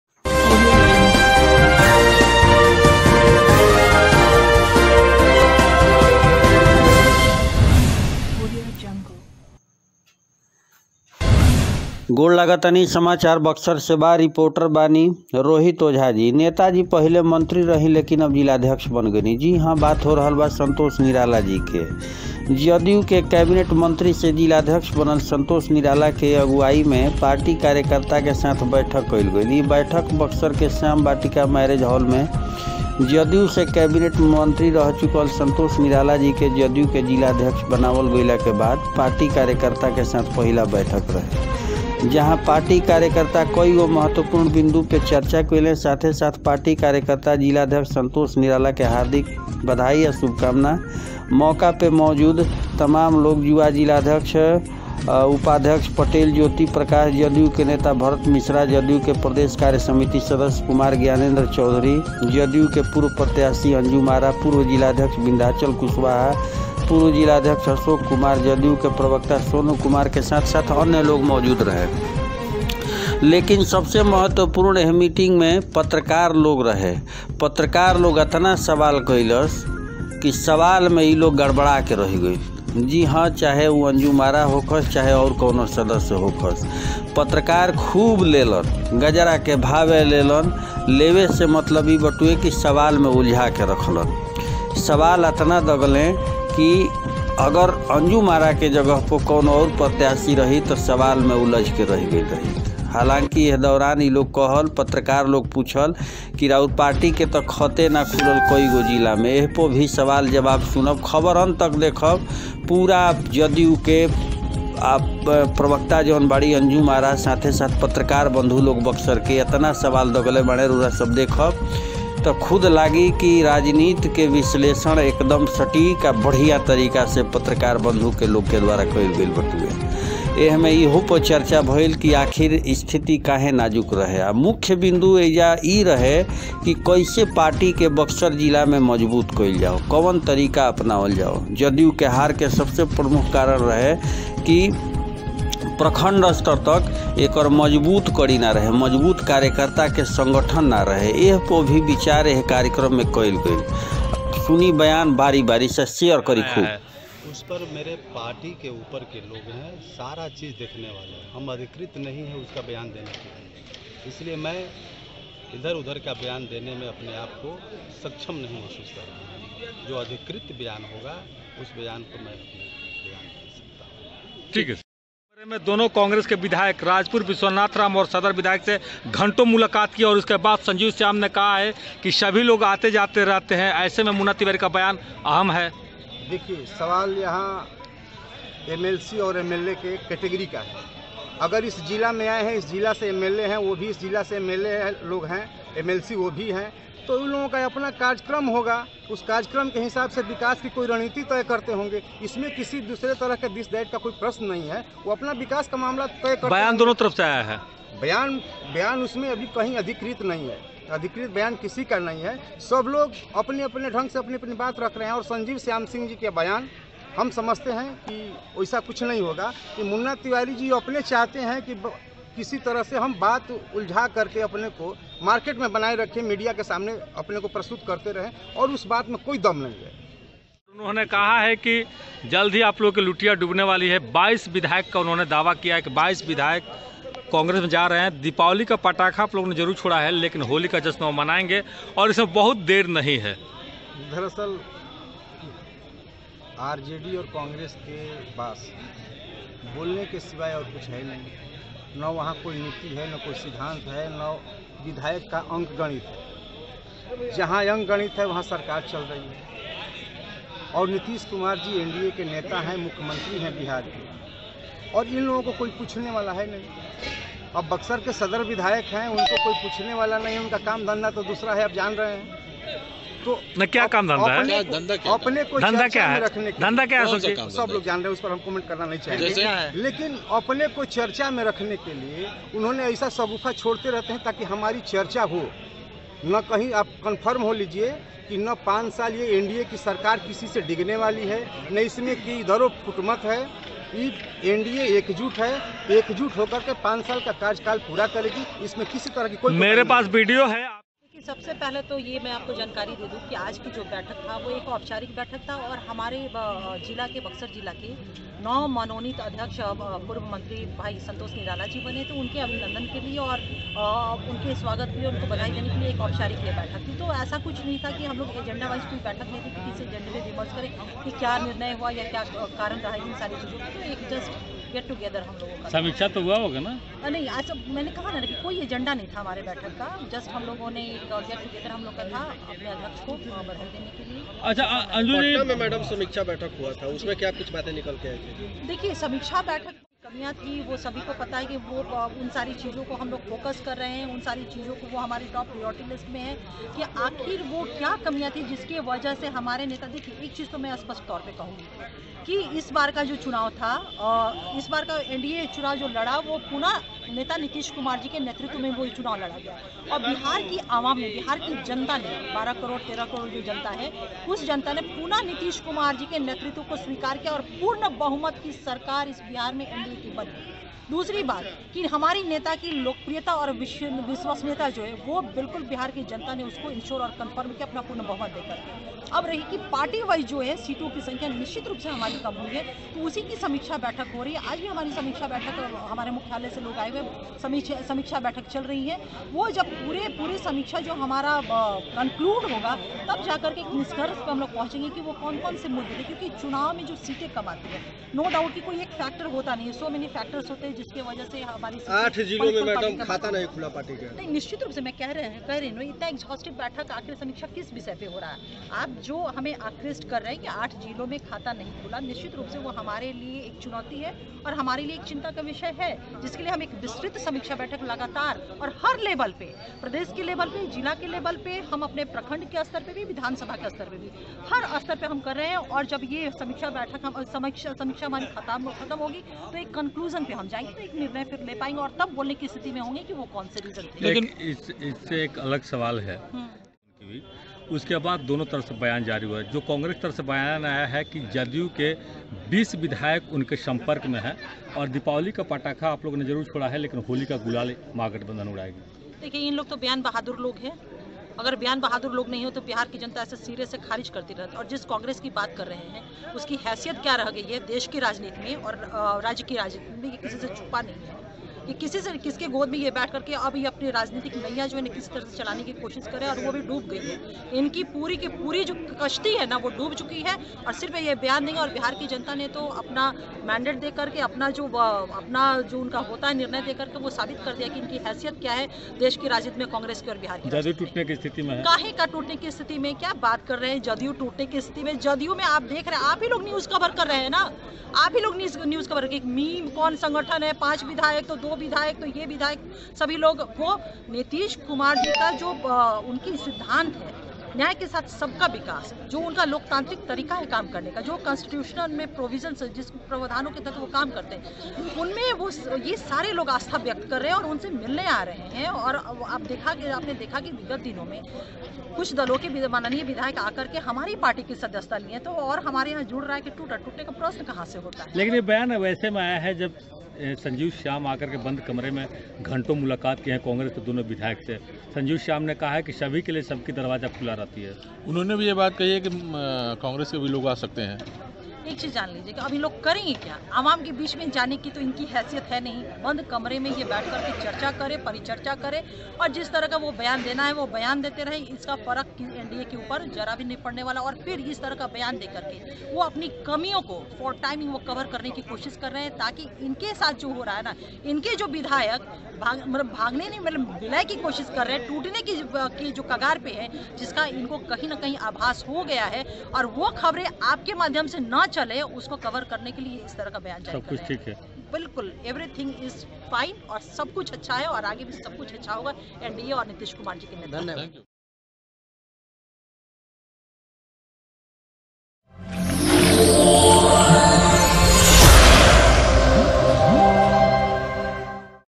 बक्सर - जदयू के कार्यसमिति के बैठक में पत्रकार बंधु लोग,लगा दिहल सवाल के लाइन,खबर शेयर करी।
श्याम उत्सव वाटिका में जदयू के कैबिनेट मंत्री से जिलाअध्यक्ष बने संतोष निराला की अगुवाई में पार्टी कार्यकर्ताओं के साथ की गई पहली बैठक
बक्सर के श्याम उत्सव वाटिका मैरेज हाल में जदयू से कैबिनेट मंत्री रह चुकी है संतोष निराला को जदयू के जिला अध्यक्ष बनाए जाने के बाद पार्टी कार्यकर्ताओं के साथ पहली बैठक की गई। जहां पार्टी कार्यकर्ताओं के द्वारा कई महत्वपूर्ण बातों की चर्चा की गई। साथी साथ सभी पार्टी के कार्यकर्ताओं ने जिला अध्यक्ष संतोष निराला को हार्दिक बधाइयां एवं शुभकामनाएं मौके पर मौजूद जदयू के युवा जिला उपाध्यक्ष पटेल ज्योति प्रकाश जदयू के नेता भरत मिश्रा जदयू के प्रदेश कार्यसमिति सदस्य कुमार ज्ञानेंद्र चौधरी जदयू के पूर्व प्रत्याशी अंजुम आरा पूर्व जिला अध्यक्ष विंध्याचल कुशवाहा पूर्व जिला अध्यक्ष अशोक कुमार जदयू के प्रवक्ता सोनू कुमार के साथ-साथ अन्य कई लोग उपस्थित रहे